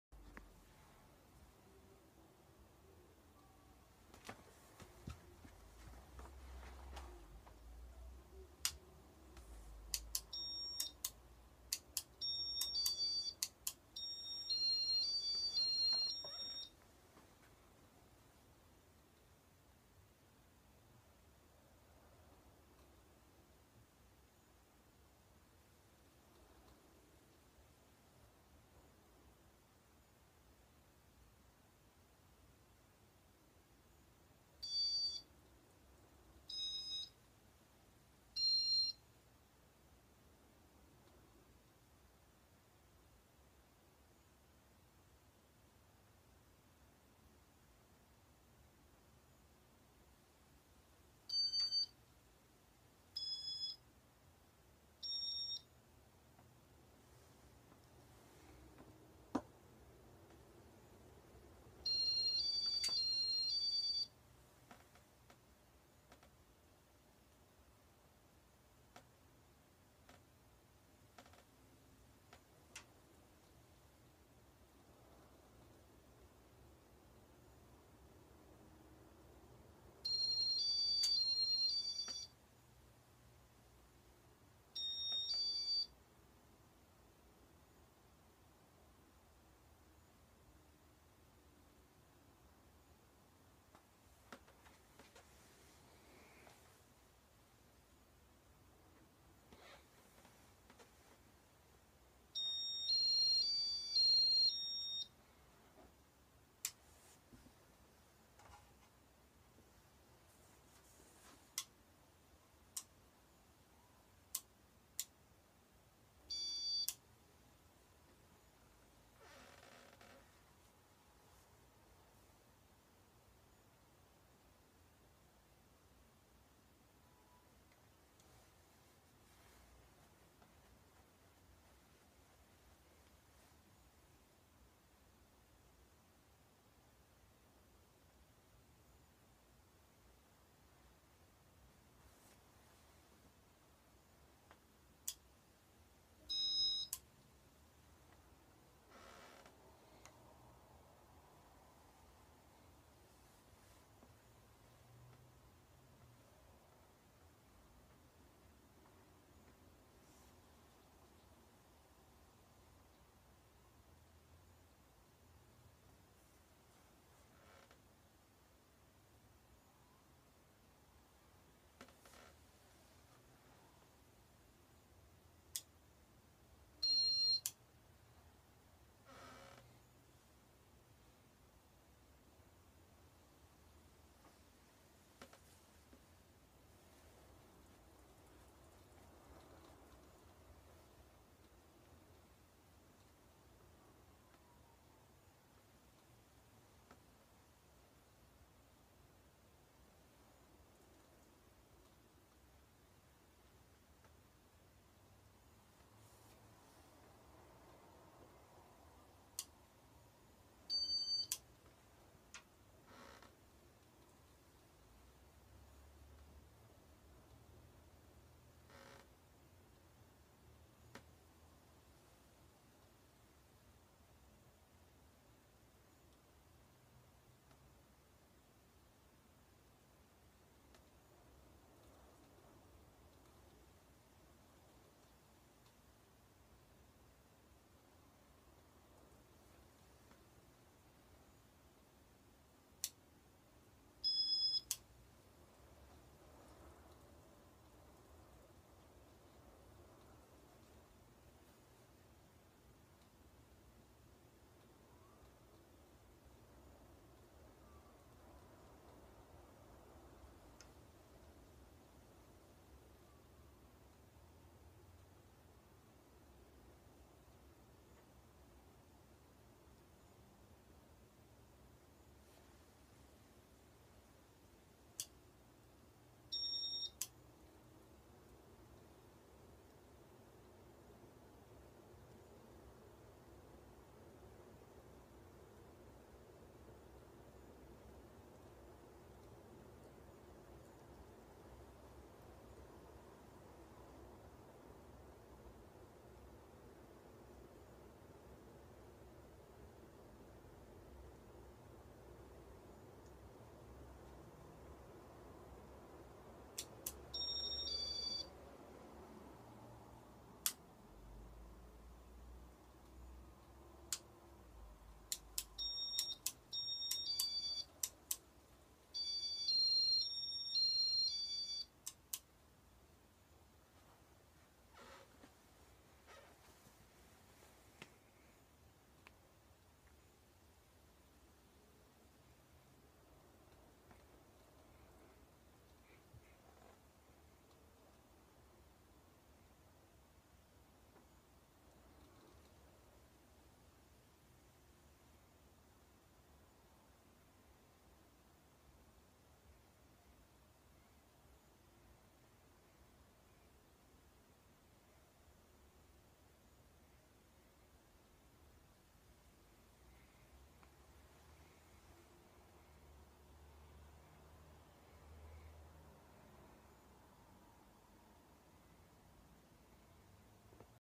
Casablanca Venus (Gallery Edition)
52" Casablanca Venus in glossy black from October 1992. I got this from Jack Conner a few days ago. Honestly, no words can describe how amazing I think this fan looks. From the large motor housing disks to the finish, it's all so cool. The gallery edition light kit is also something to be in awe in. I've never seen a light like this but now that I have, I can't stop looking at it lol. Truly a stunning piece of art. Controlled by a W11 and has a 5 blade only disk that mounts to the flywheel